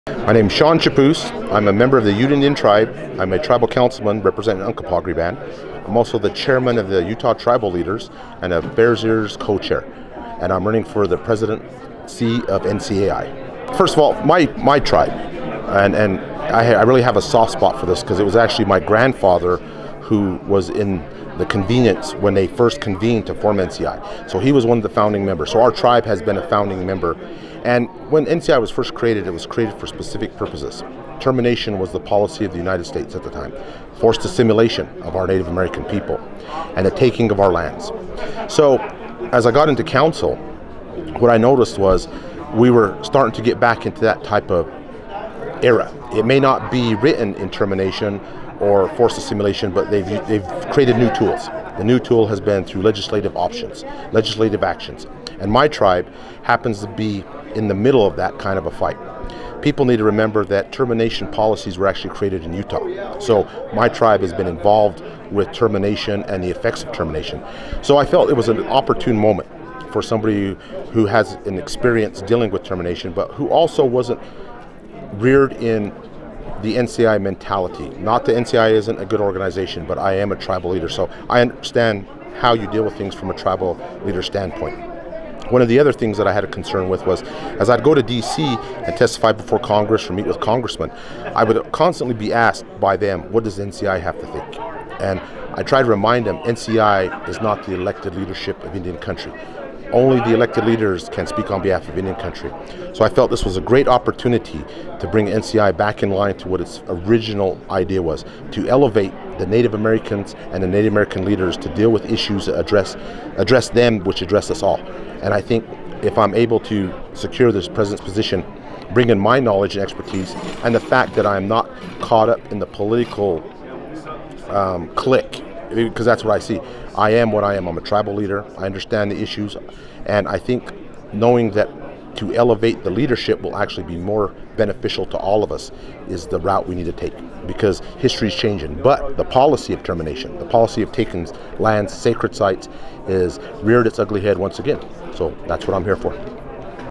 Shaun Chapoose | National Congress of American Indians
"Only the elected leaders can speak on behalf of Indian Country": Shaun Chapoose, Ute Tribe, is running for President of the National Congress of American Indians. Learn more about his candidacy as NCAI prepares to elect a new set of leaders during its 76th annual convention in Albuquerque, New Mexico.